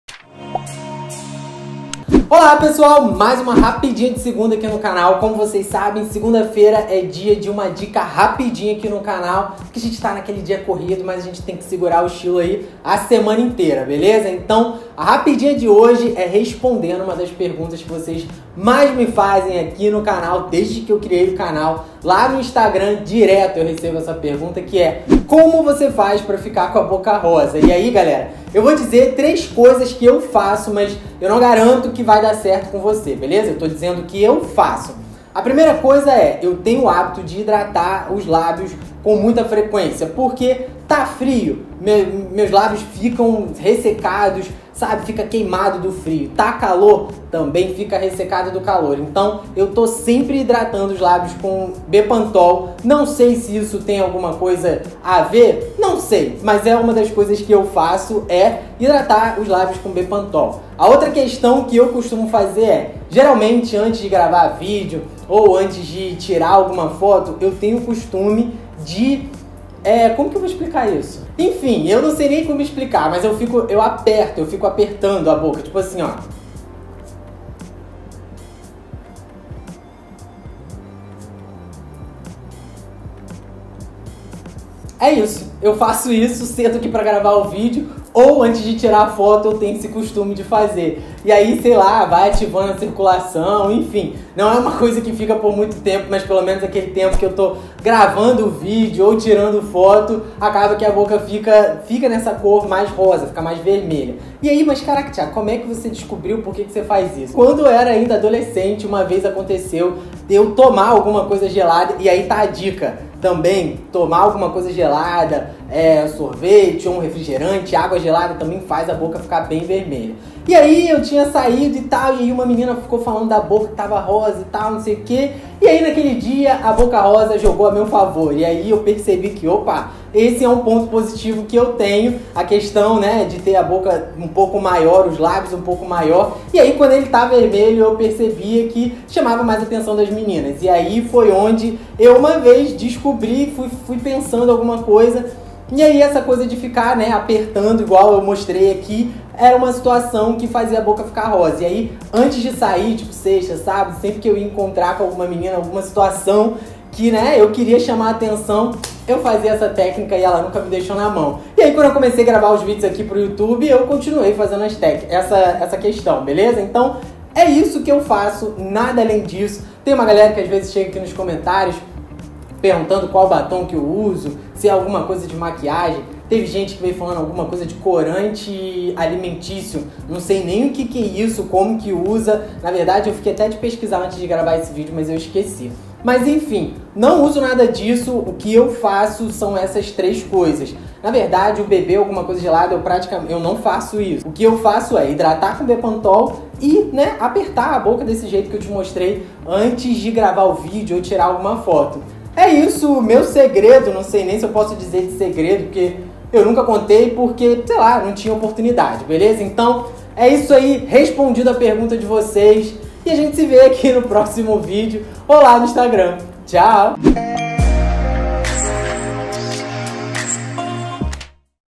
Como ter a BOCA Vermelha e Rosada Natural | Rapidinha de Segunda
Como ficar com a BOCA ROSA. Recebo essa pergunta desde o primeiro vídeo aqui no canal. Então resolvi contar pra vcs o meu segredo para ter a boca rosa sem usar maquiagem.

VOCÊ TAMBÉM ME ENCONTRA POR AQUI
Facebook ► Thiago Martins de boa na moda
Facebook Grupo ► “Personal Stylist – Moda masculina”